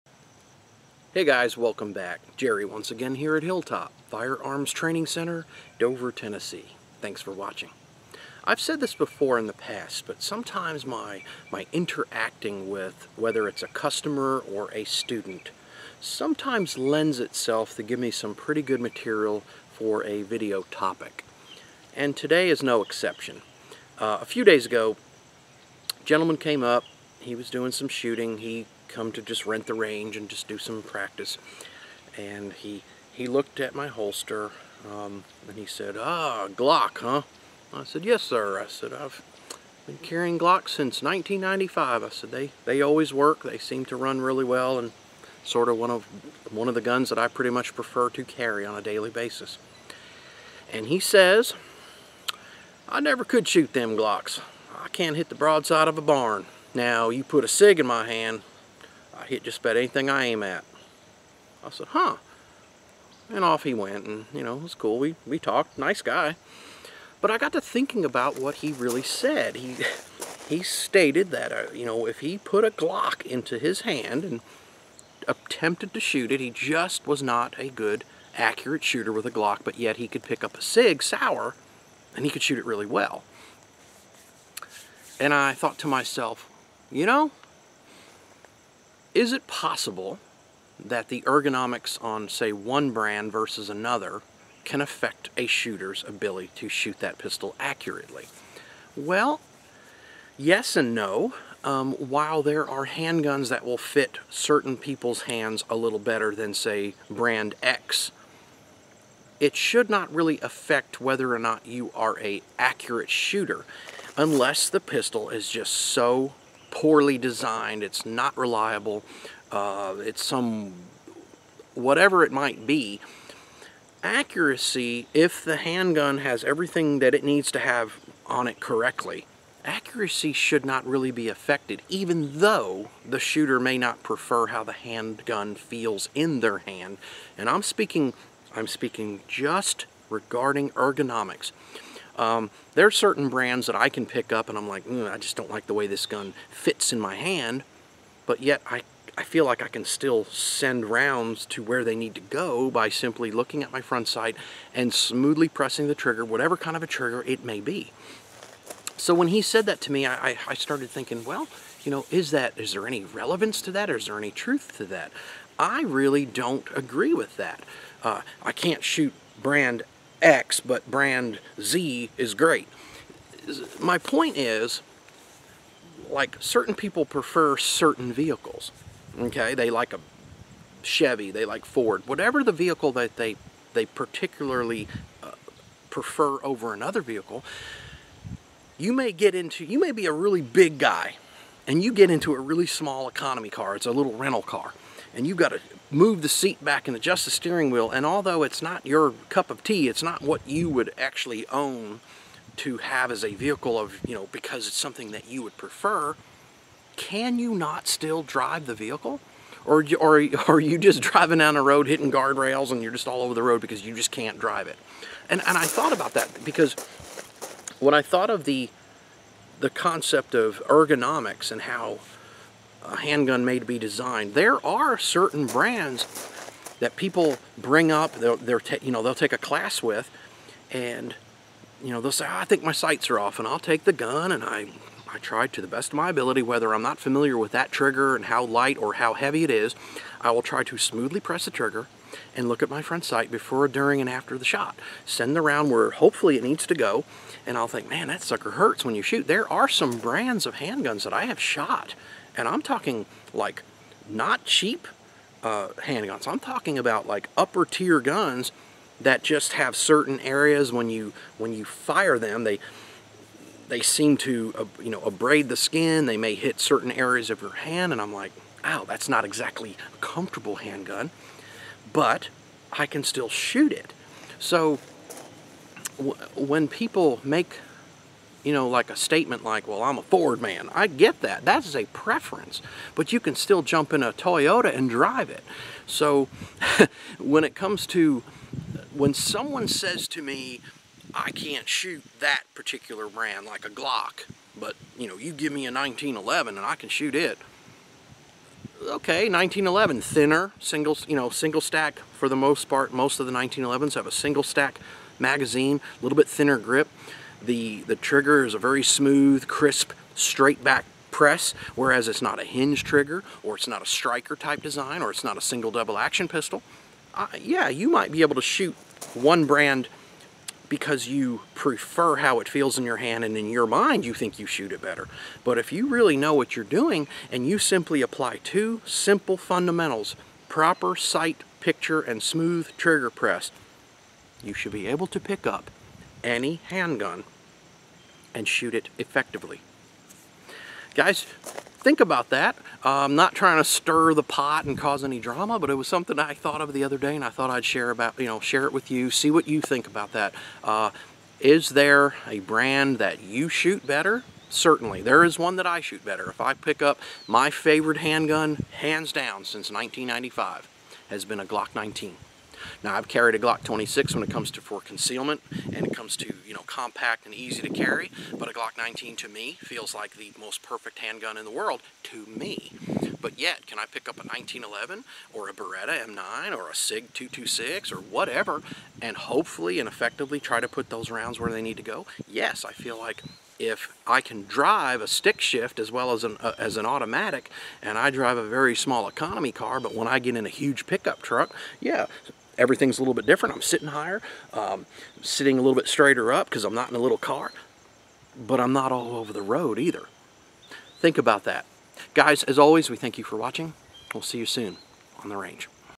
I can't shoot a Glock!
Can a person make the claim they can only shoot a certain brand handgun such as Sig, yet are unable to shoot accurately, a Glock pistol for example.
Fundamentals are what determine what we are able to do when shooting, despite the brand.